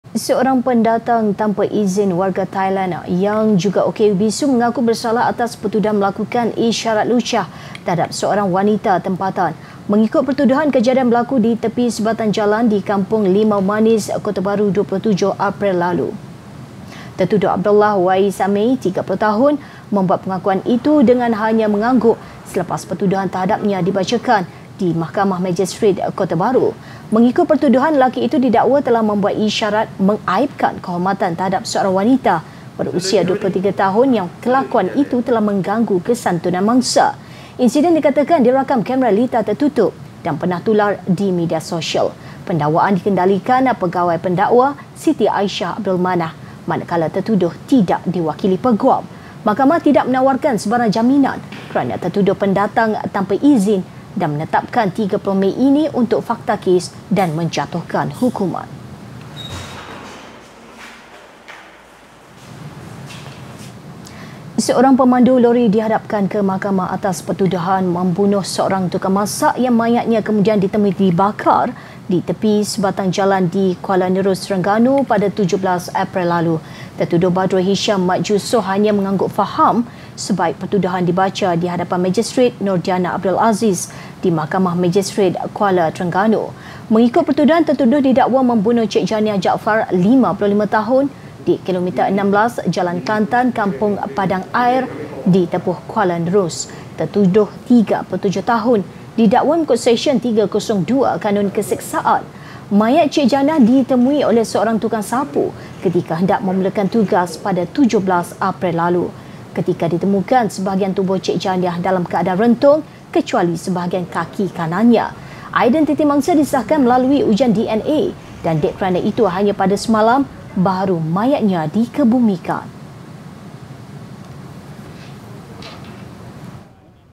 OKU Thailand Mengaku Lakukan Isyarat Lucah
Seorang pendatang tanpa izin, warga Thailand mengaku bersalah atas pertuduhan melakukan isyarat lucah terhadap seorang wanita tempatan.